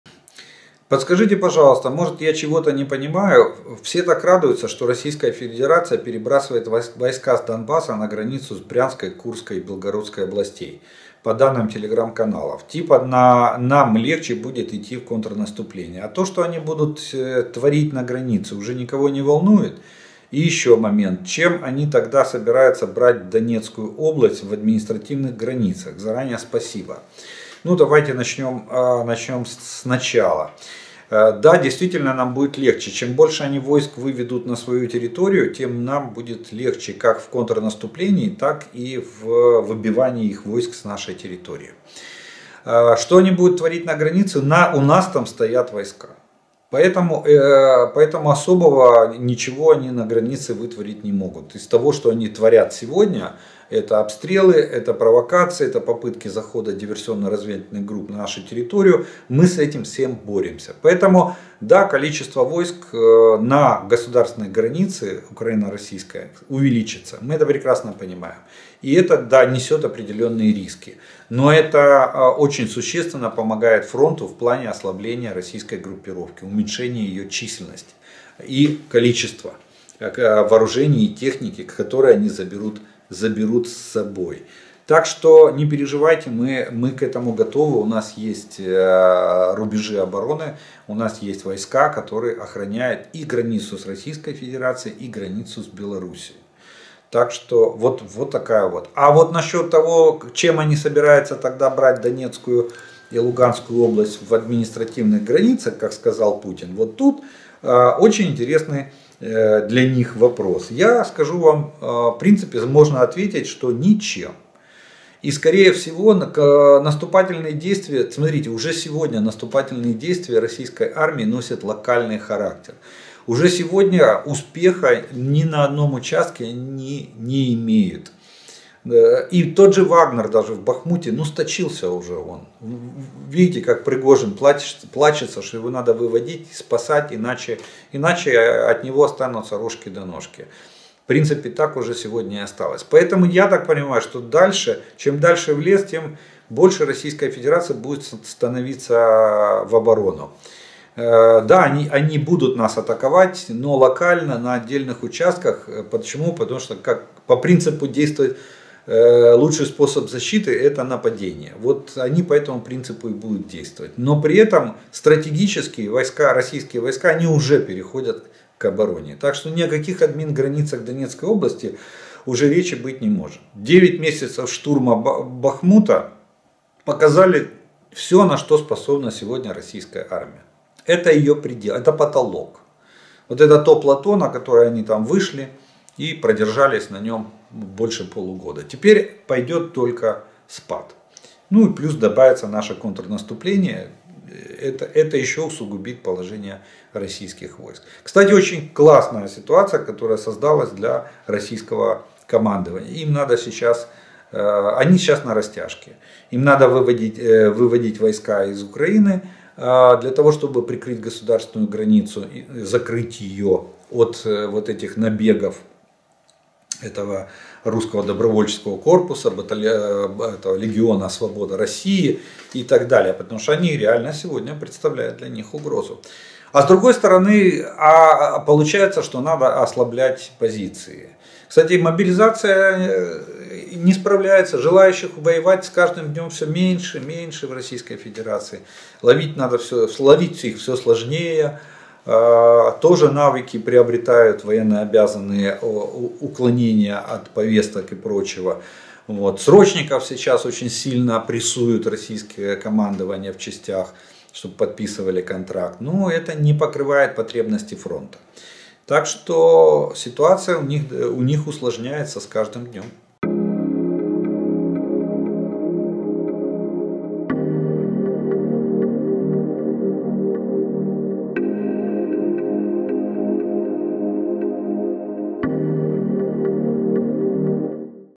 Россия выводит войска из Украины для обороны Белгорода: Жданов объяснил
UA - Військовий експерт Олег Жданов (@OlegZhdanov) пояснив, що змінить на фронті часткове виведення російських військ з України на "проблемну" ділянку кордону.

RU - Военный эксперт Олег Жданов (@OlegZhdanov) объяснил, что изменит на фронте частичный вывод российских войск из Украины на "проблемный" участок границы.